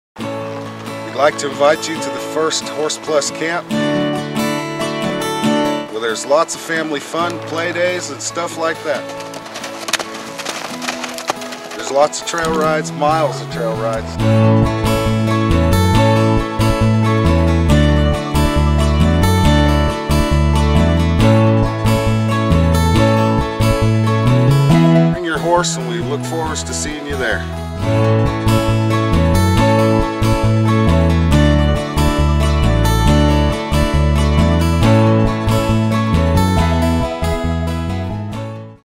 Horse Camp 2013
We would like to invite you to the 2013 Horse Plus Humane Society Extreme Rescue Makeover, Horse Camp and Play Day May 26 and 27 in Taylorsville, California.